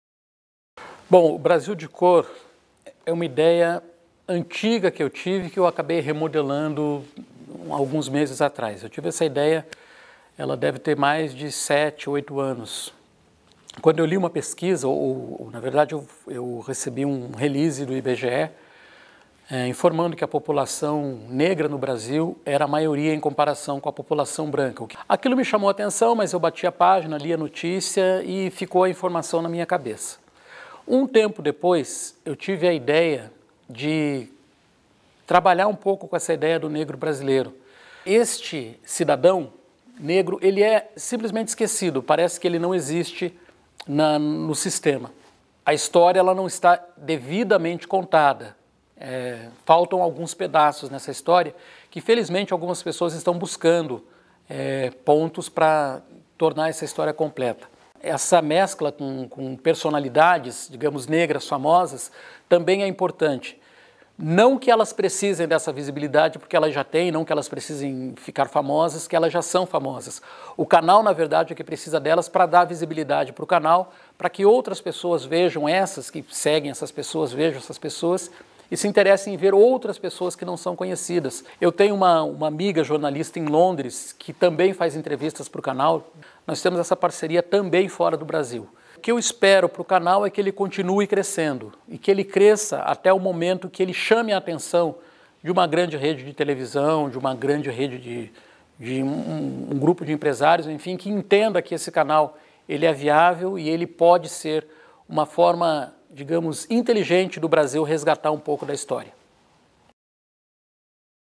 Comunidade negra de Curitiba se fortalece - Herivelto Oliveira | Portal Comunicare
Movimentos dão visibilidade para população negra da capital paranaense.